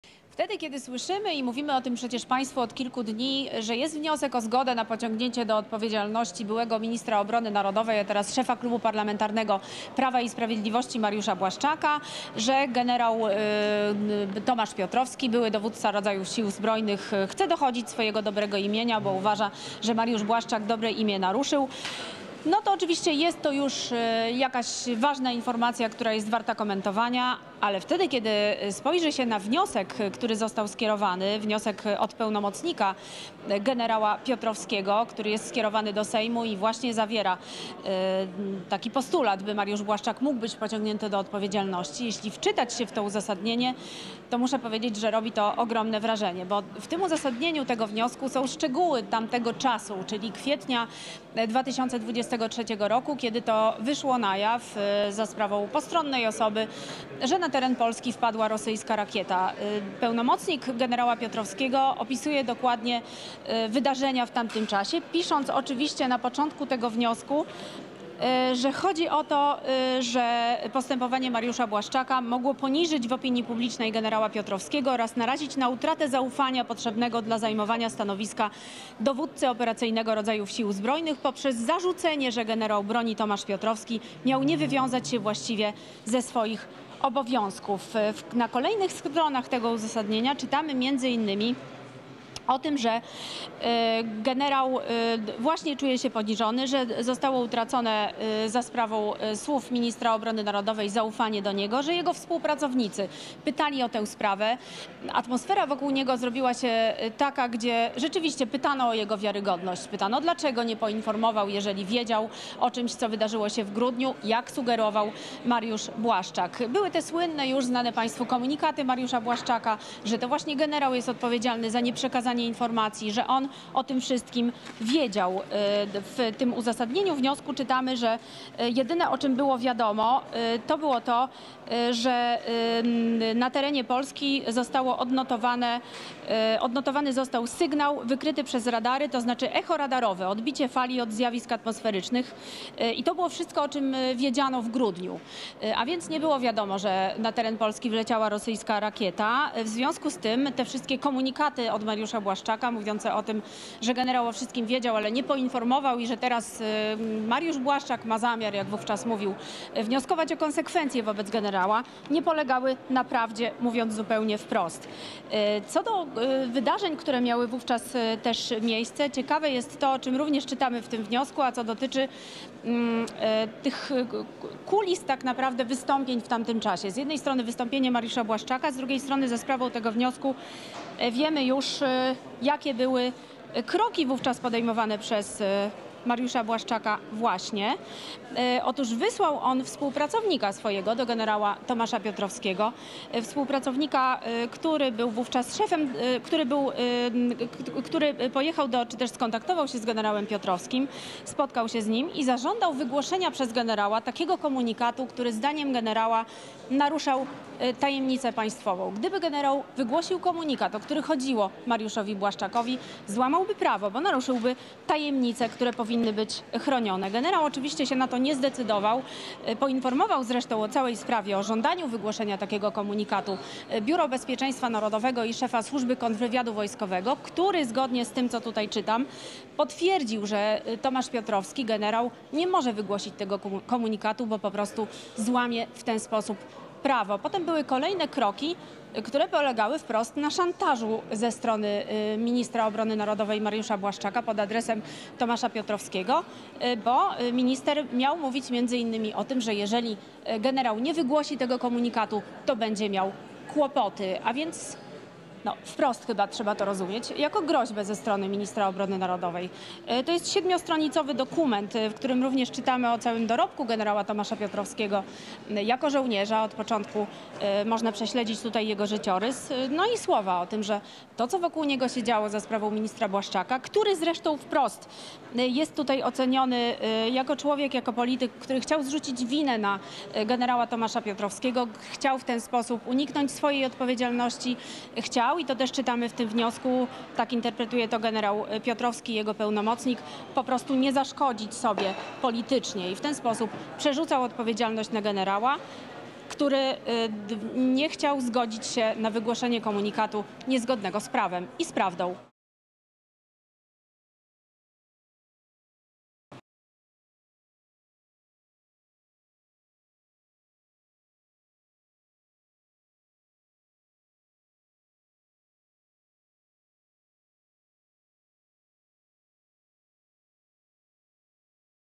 Wniosek o uchylenie immunitetu Błaszczaka. Reporterka TVN24 dotarła do treści dokumentu
Do treści uzasadnienia wniosku o uchylenie immunitetu Mariuszowi Błaszczakowi dotarła reporterka TVN24 Agata Adamek. Jak mówiła, zarzuca się w nim Błaszczakowi, że w wyniku jego działań generał Tomasz Piotrowski "czuje się poniżony" i że "zostało utracone zaufanie do niego". W uzasadnieniu wniosku mowa też o tym - wskazywała dziennikarka - że były szef MON za pośrednictwem swojego współpracownika "zażądał wygłoszenia przez generała takiego komunikatu, który zdaniem generała naruszał tajemnicę państwową".

zdjęcie na okładce: PAP/Art Service